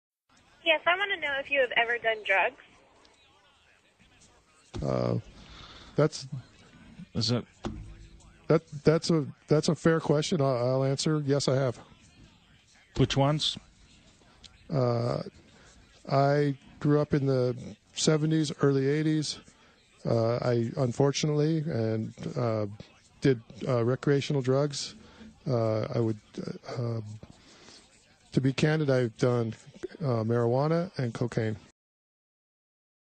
Pete Domenici Admits Cocaine Use
Pete Domenici Jr admits using the "recreational drug" cocaine.